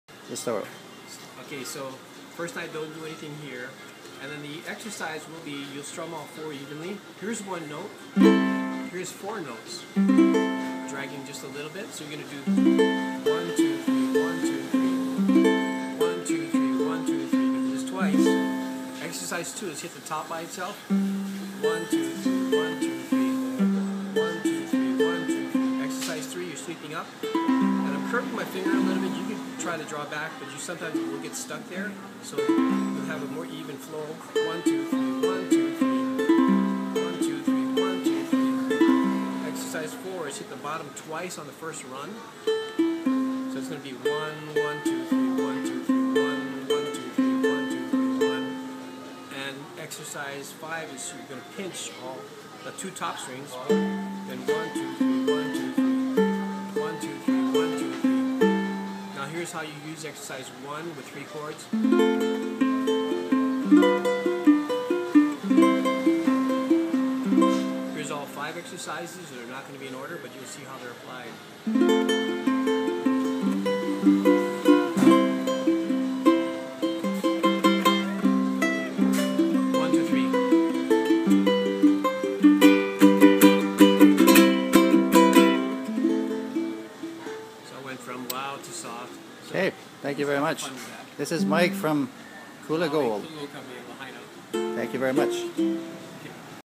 Ukulele demo in Maui
Quick demonstration received by Mike Dougherty of Kula Gold Co. in Lahaina, Maui, Hawaii when I purchased my ukulele.